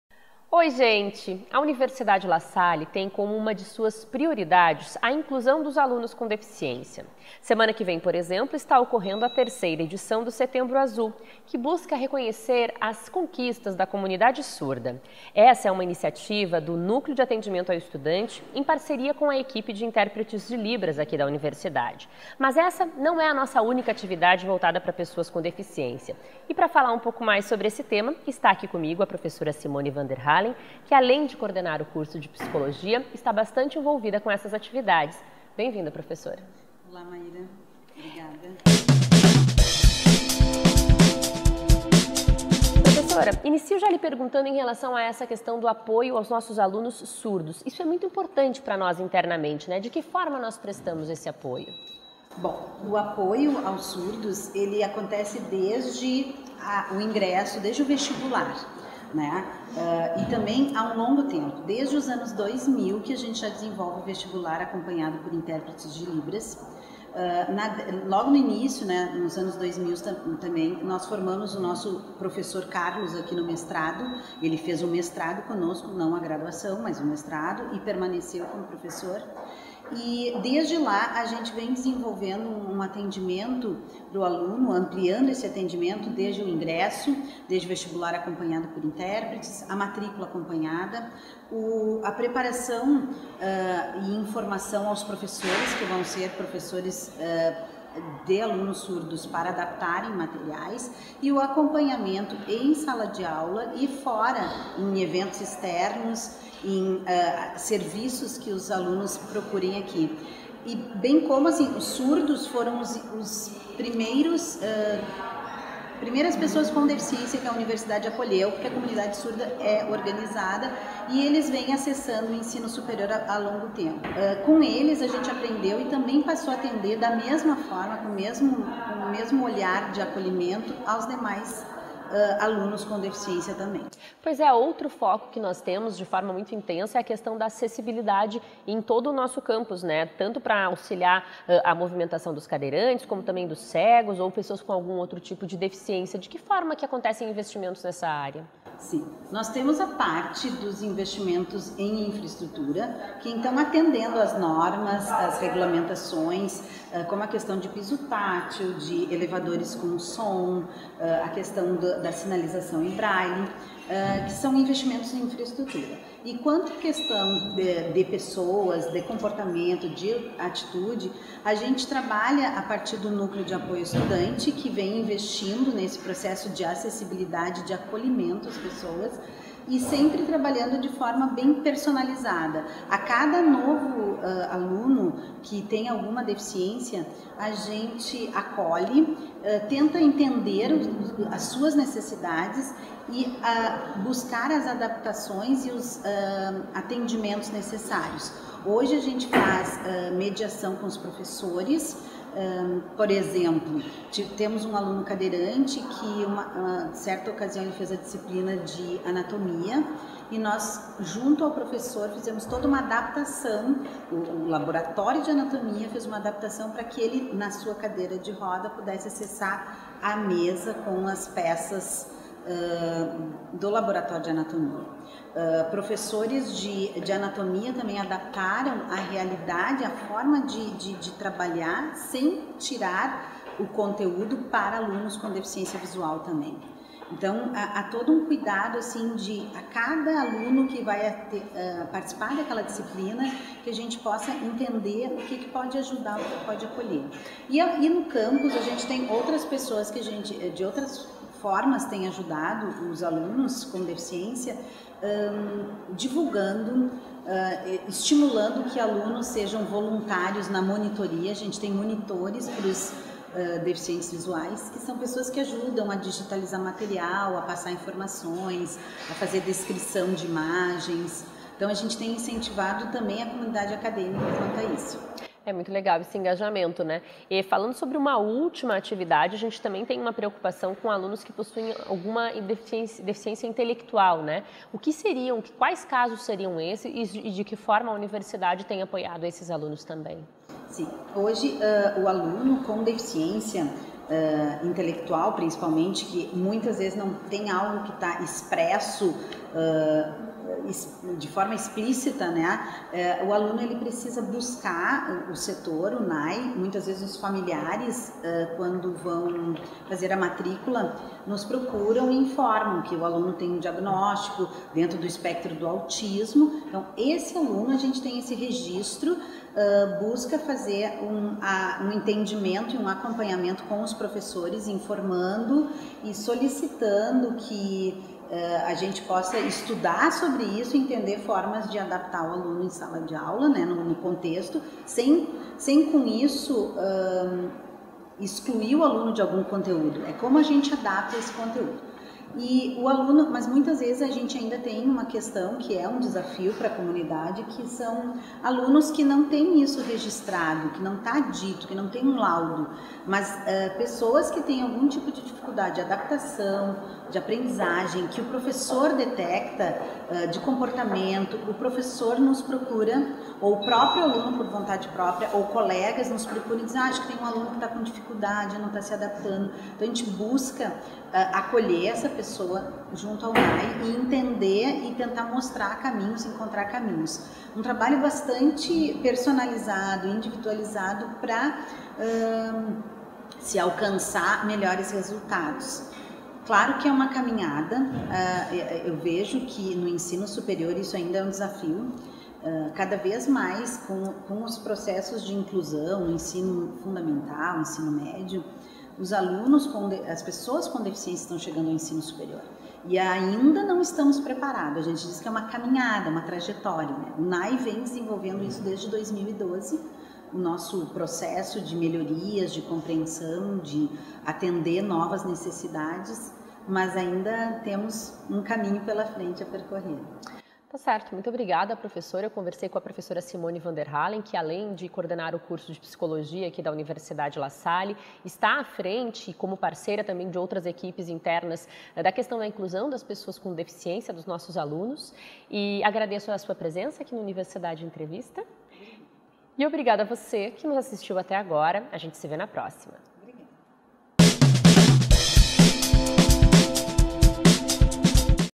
Universidade Entrevista - Professora Simone Van Der Halen
A Universidade La Salle tem diversos projetos que trabalham a inclusão dos alunos que têm alguma deficiência, mas sabemos que essa é uma caminhada constante. A Prof. Me. Simone Van Der Halen explicou no Universidade Entrevista mais sobre essas atividades e os desafios que ainda temos pela frente.